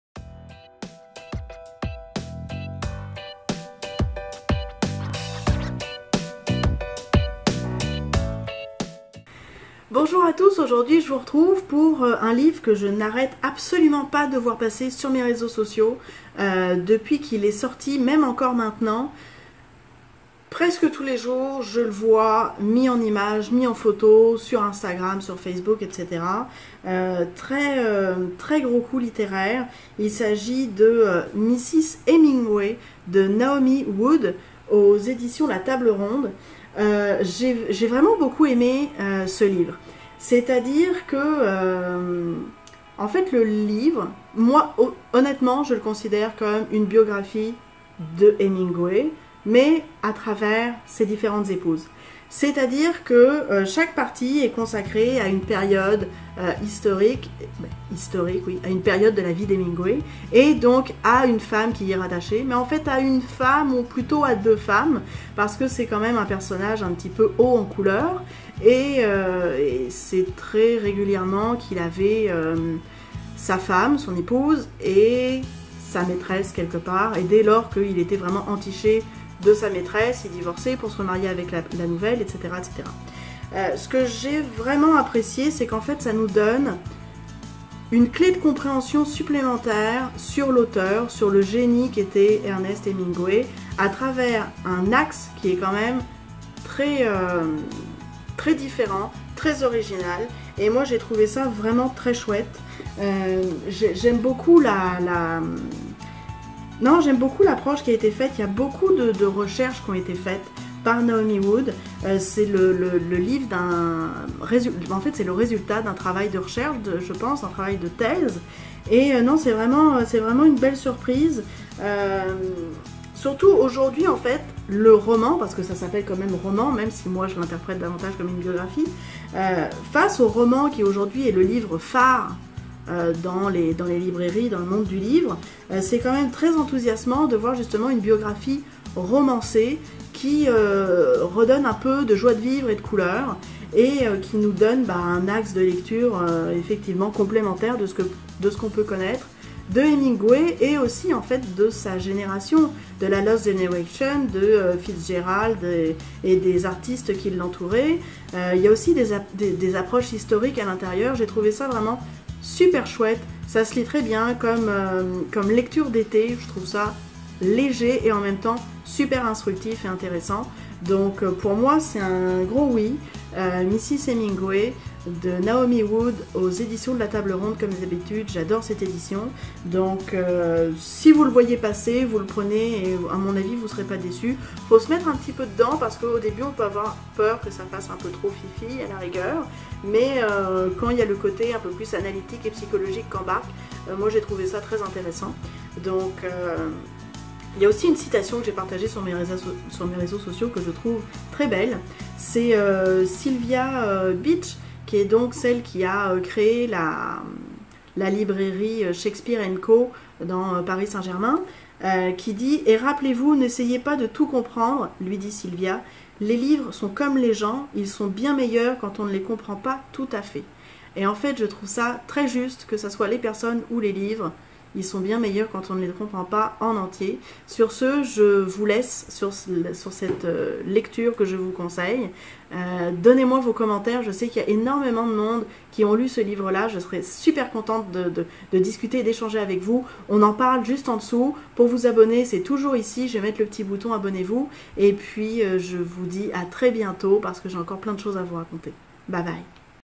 Mrs Hemingway de Naomi Wood - Update Lecture
Dans cette vidéo, je vous partage un livre qu'on voit passer absolument PARTOUT :

MRS HEMINGWAY
De Naomi Wood aux Éditions de la Table Ronde

Les musiques que j'utilise sont toutes libres de droit.